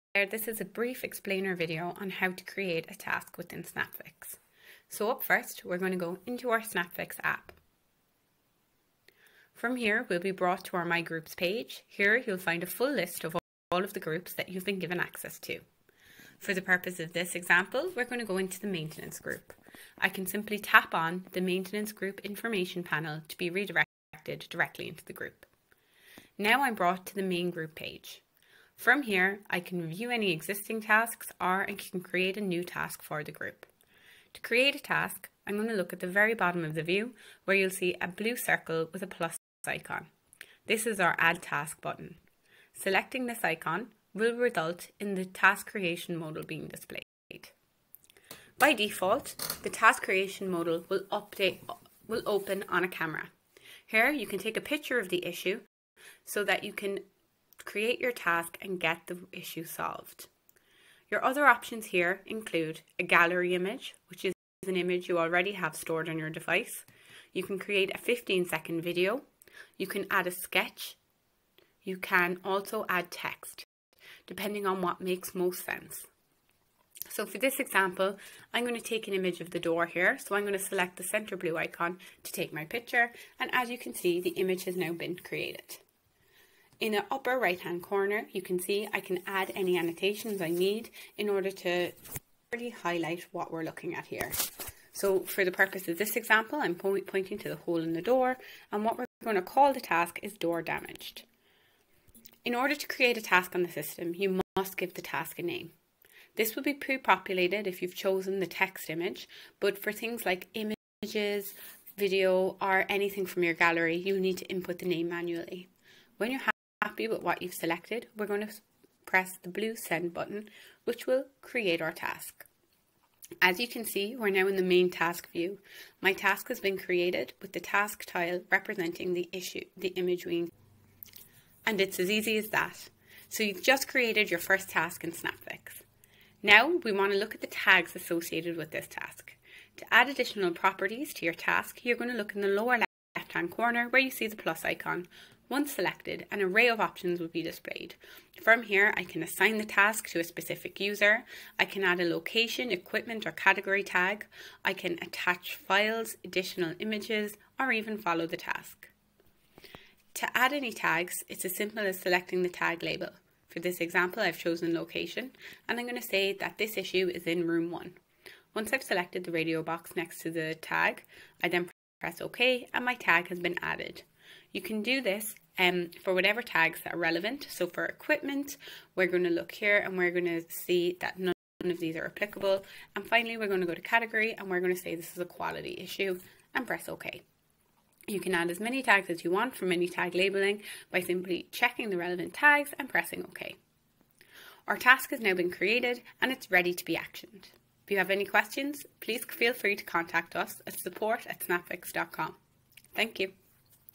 Snapfix - How to Create a Task
In this tutorial, we will show you how to create a new task. You can choose to start a task with either a photo, video, or text. Tasks are created once a task is submitted, tags can then be added and users can be assigned. Make sure to visit our support center on snapfix.com for more helpful resources.